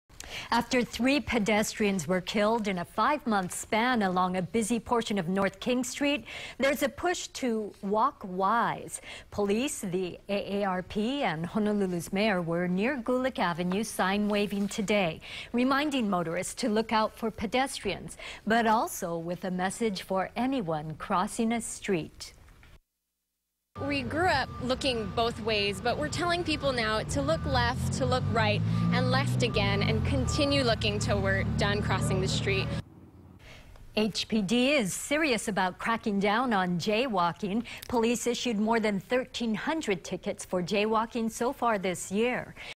Pedestrian safety emphasized in sign-waving campaign
Police, AARP and the mayor were out sign-waving Tuesday, reminding motorists to look out for pedestrians and urging pedestrians to be careful when crossing the street.